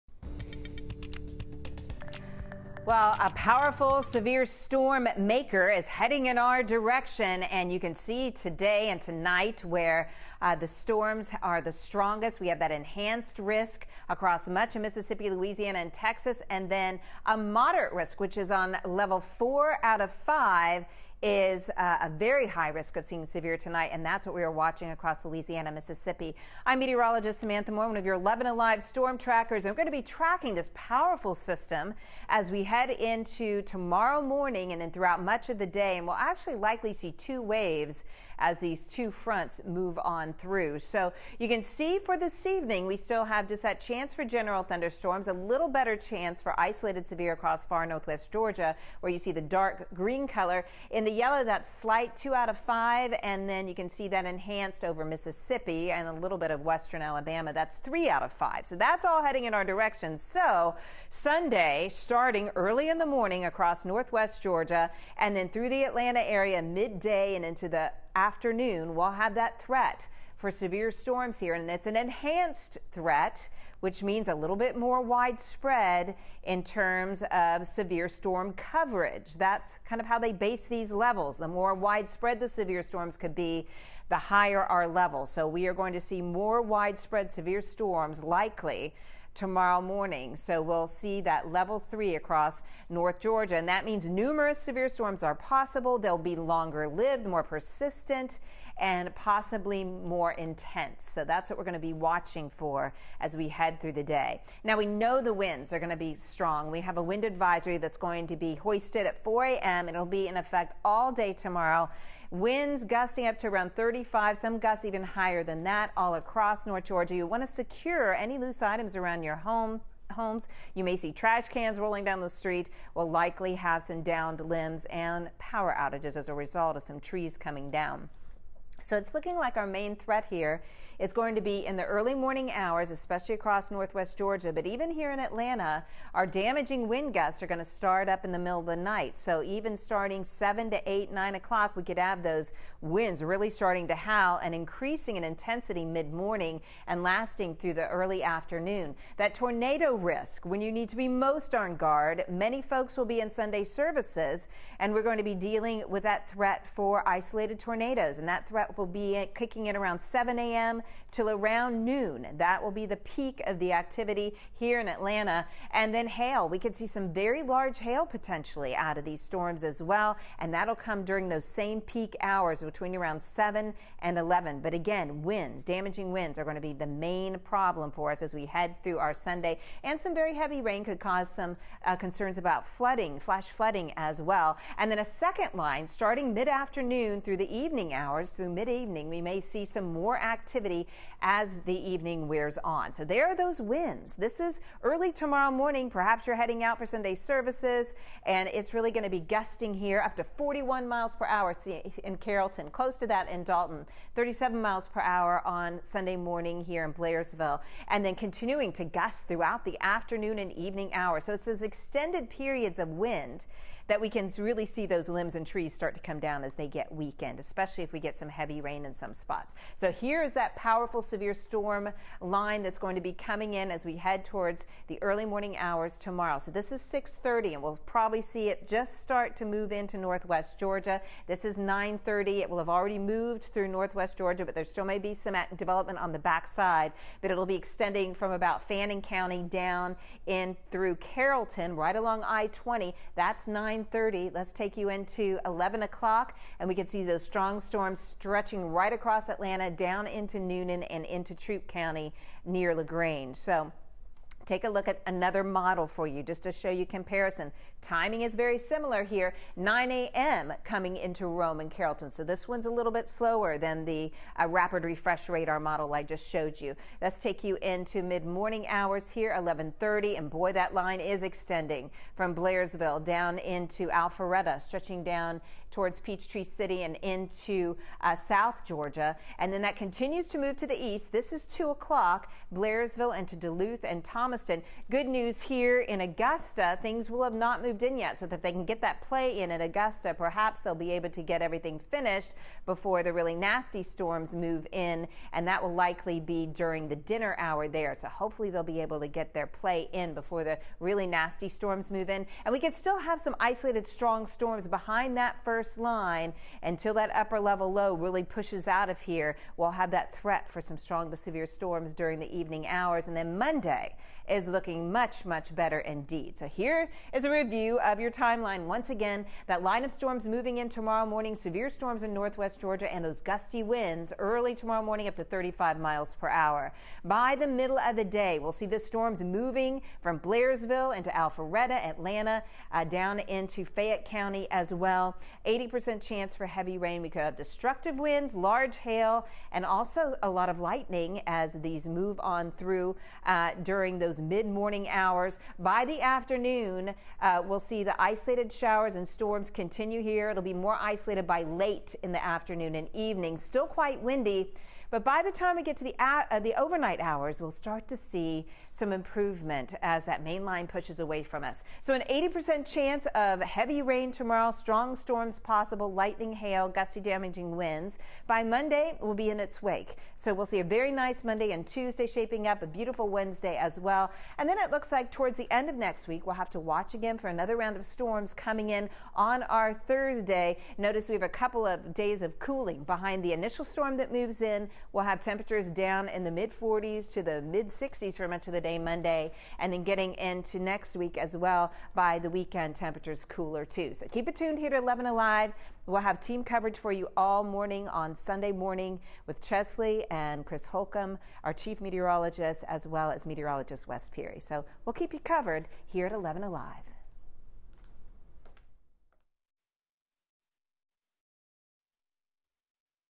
Severe weather moving into Georgia on Sunday
Sunday morning brings threat for severe storms.